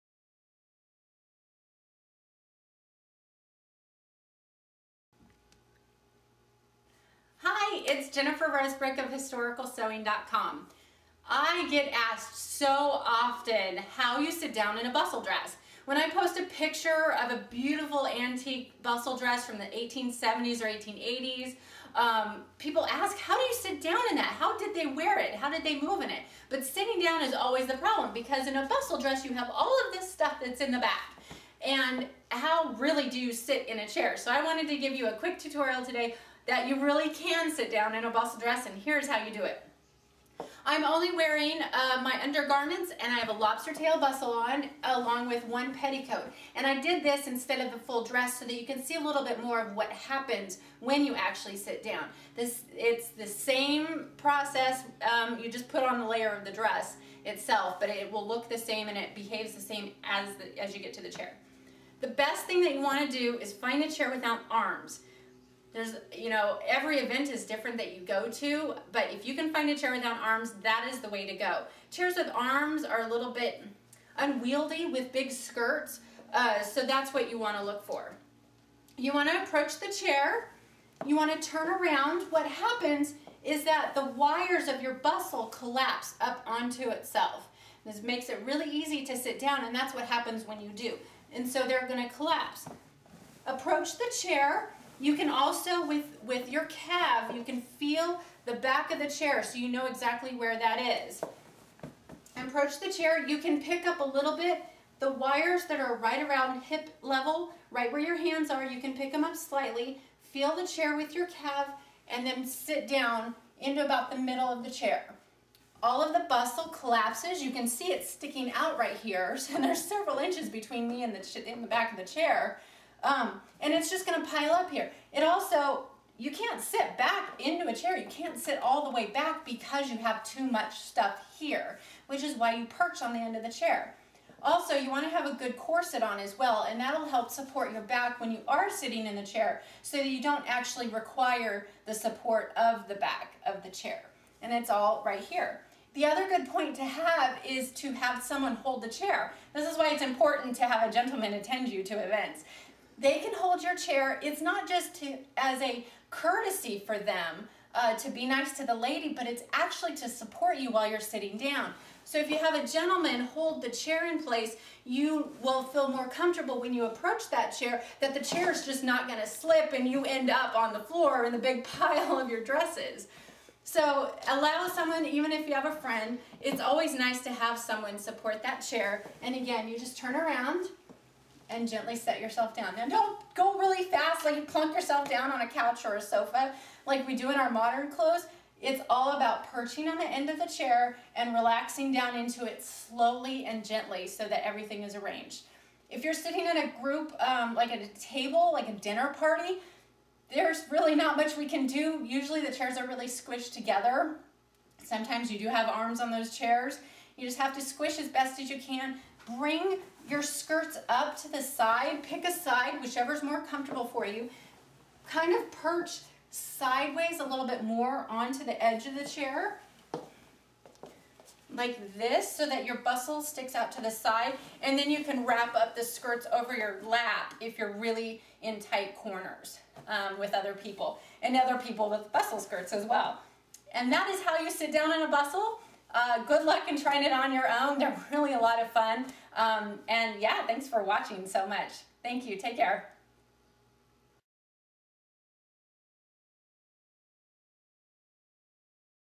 How to Sit in a Victorian Bustle
For anyone who is perplexed about how Victorian women (or those of us who dress in Victorian clothing) sit down while wearing a large wired bustle and petticoats.